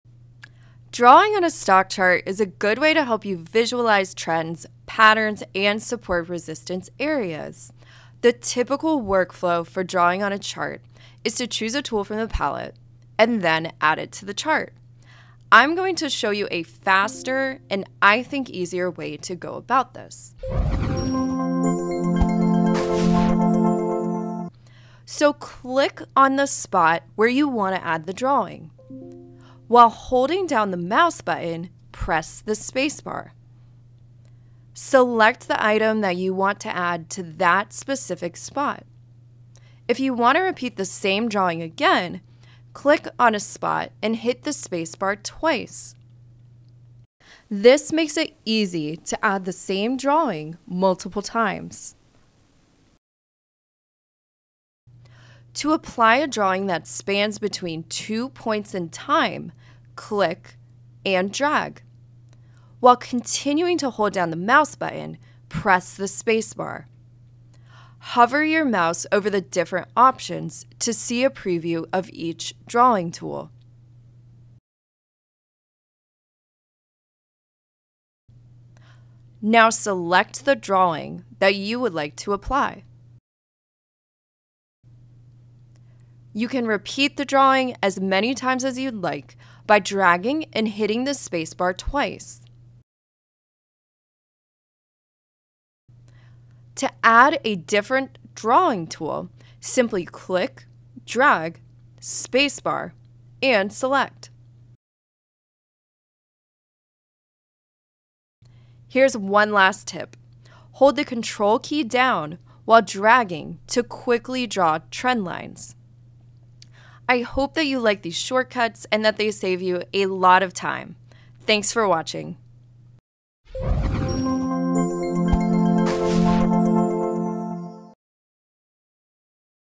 Drawing Shortcuts in TC2000
Learn a simple, time-saving workflow that unlocks the entire library of drawing tools in your charts. You’ll discover how to quickly preview each tool and see how it fits into your chart before you commit. It's a great way to become familiar with the tools that you never would have used. We’ll also show you the quickest way to draw trendlines and effortlessly repeat drawings.

Shortcuts covered in the video:

To preview markers: Left-click on chart, hold down mouse button, press spacebar.
To preview drawing tools: Left-click and drag on chart, hold down mouse button, press spacebar.
To repeat a marker: Left-click on chart, hold down mouse button, press spacebar twice.
To repeat a drawing tool: Left-click and drag on chart, hold down mouse button, press spacebar twice.
To quickly draw a trendline: Ctrl + left-click and drag on chart.

00:00 Introduction
00:20 Add markers such as arrows
00:39 Quickly repeat markers
00:54 Preview drawing tools anywhere on chart
01:24 Quickly repeat drawing tools
01:35 Recap drawing tool workflow
01:48 Fastest way to draw trendlines